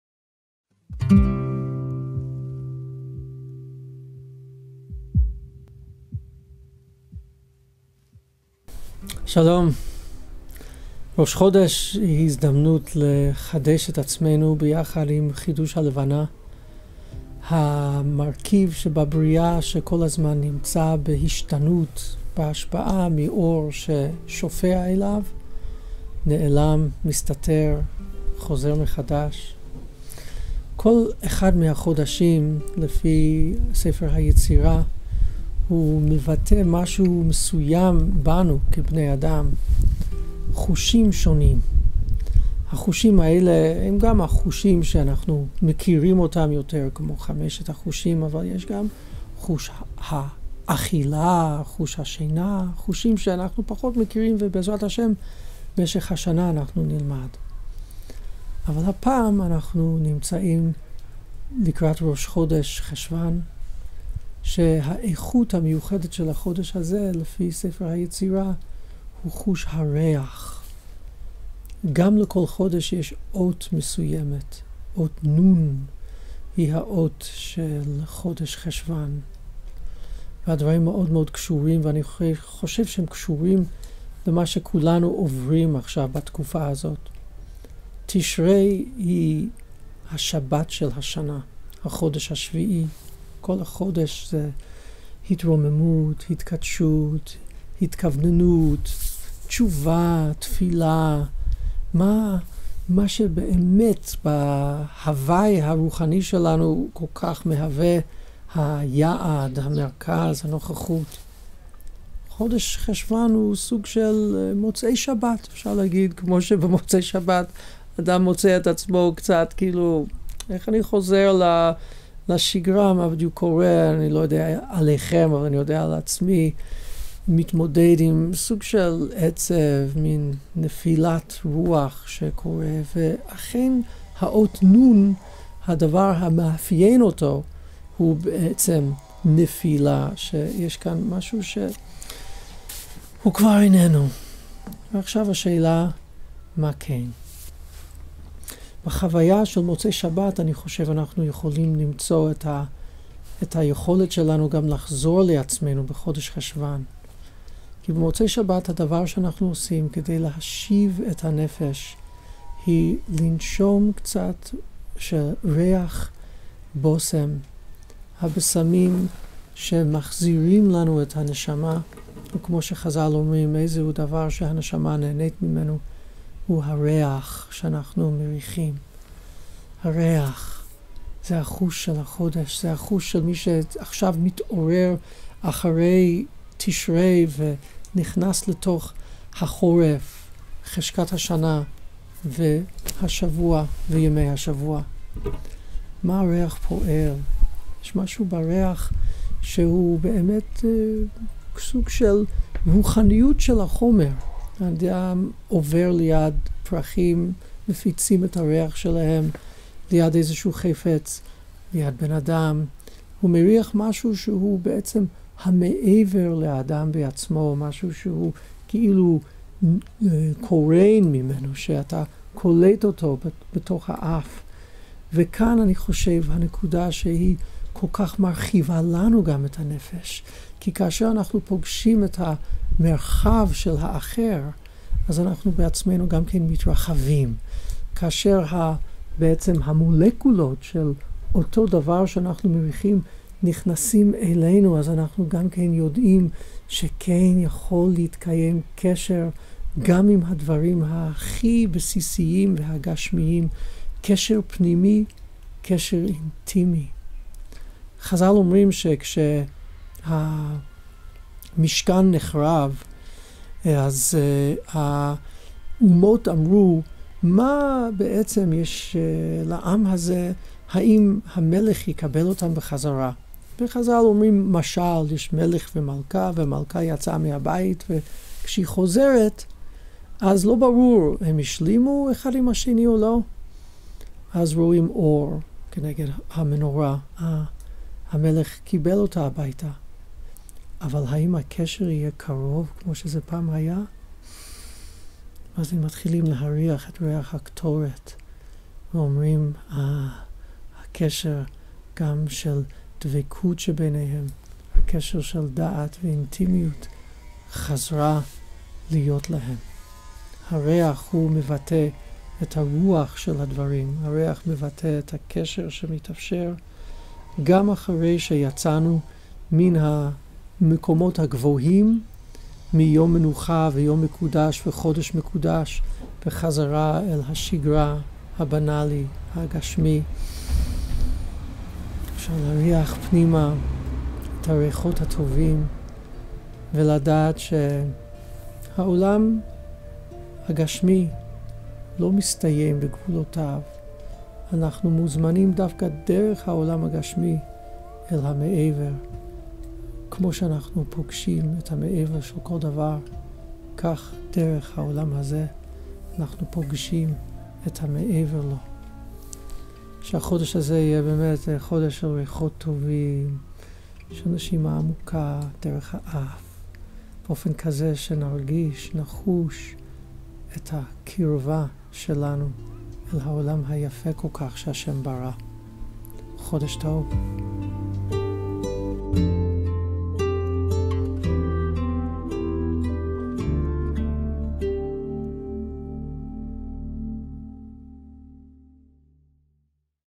חשוון-ריח ניחוח לנופלים ברוחם
חשוון, לפי ספר היצירה, קשור לחוש הריח.
חוש הריח מחזיר אותנו אל חיי היום יום
בדרך מיוחדת המשיבה את הנשמה.